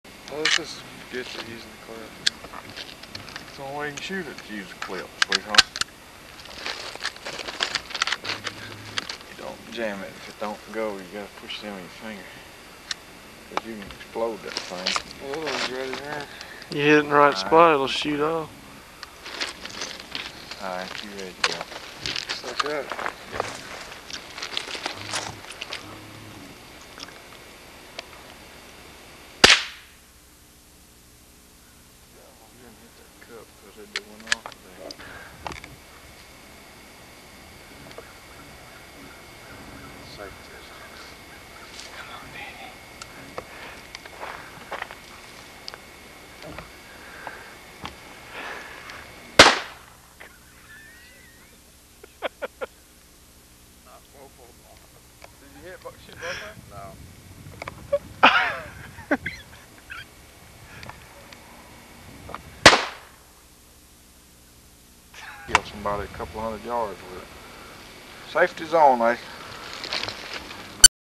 Asa Shooting Guns - 1990 00001
Asa and Daddy Shooting Shotguns